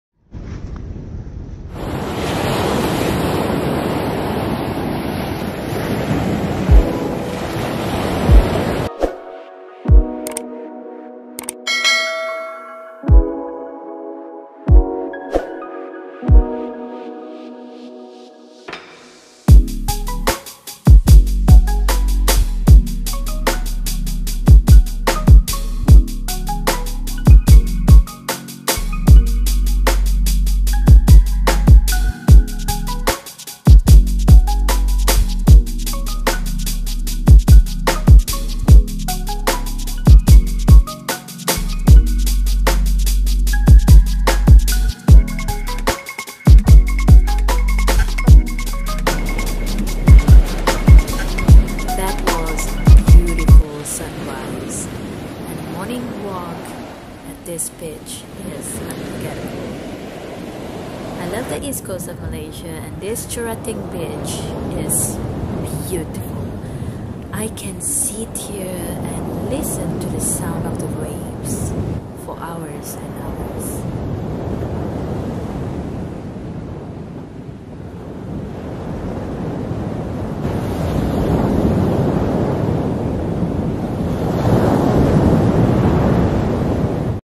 Cherating Beach - Pahang
I went to these beautiful beach in eastcoast part of Malaysia n enjoying the beautiful sound of the waves n sunrise.